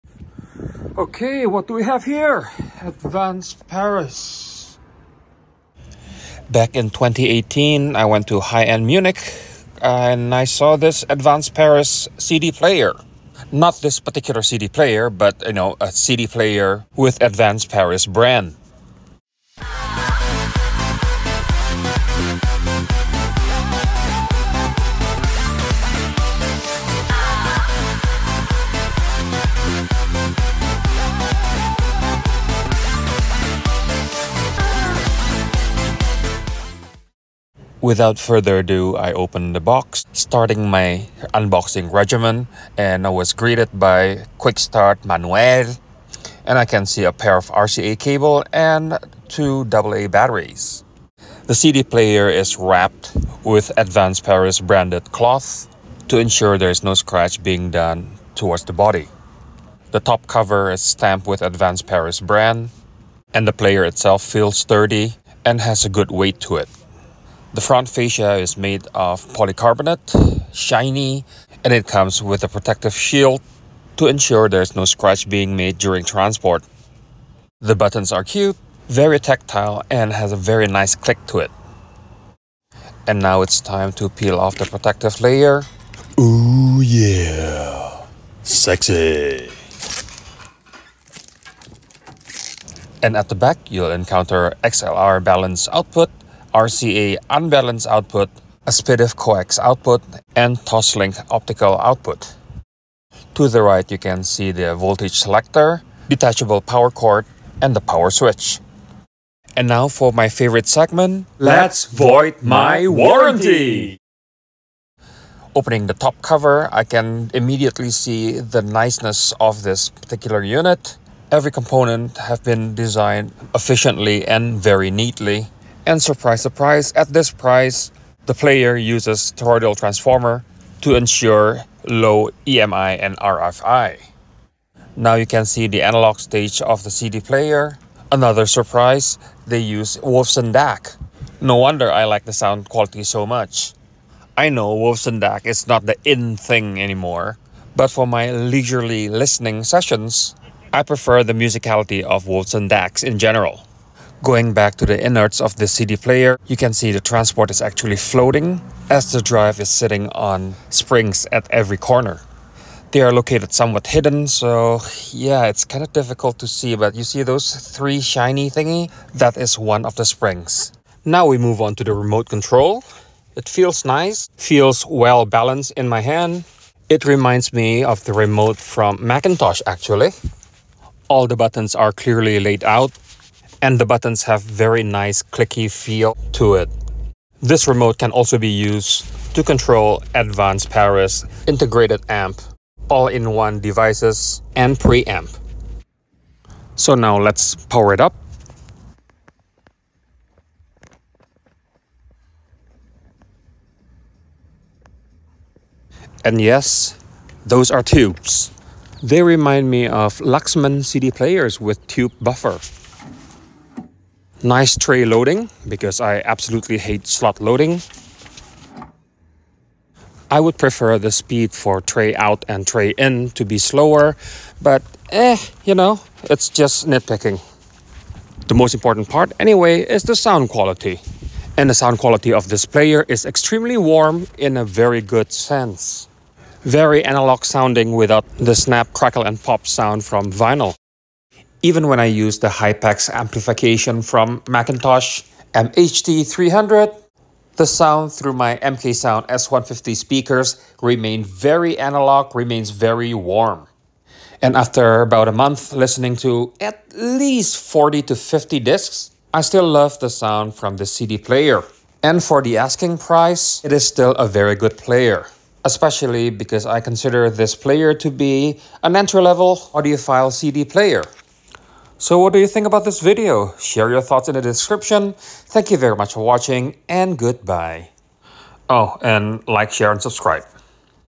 Advance Paris X-CD9 Audiophile Tube CD Player Review CD9 Wadia Luxman McIntosh Attributes Combined
Luxman, McIntosh, Wadia combined - Advance Paris X-CD9

Discussing the budget audiophile cdplayer and how it fares

Priced at C$1,299 CAD
Tricell Enterprises
391 Hanlan Road
unit 4
Woodbridge, Ontario
L4L 3T1
Canada